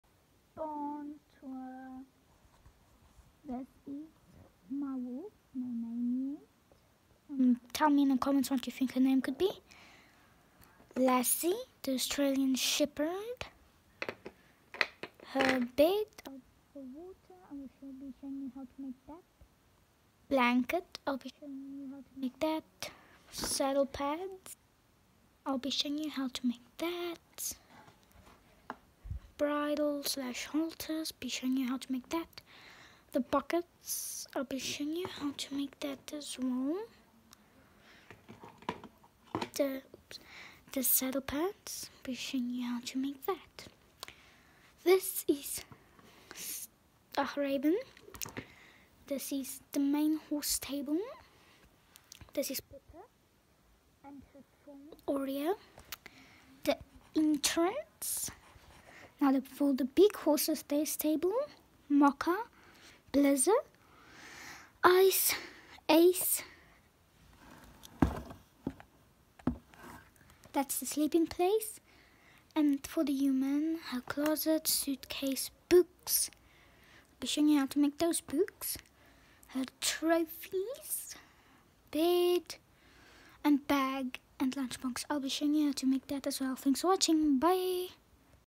Barn Tour 2020
This is my stables and tack room and everything I will be making.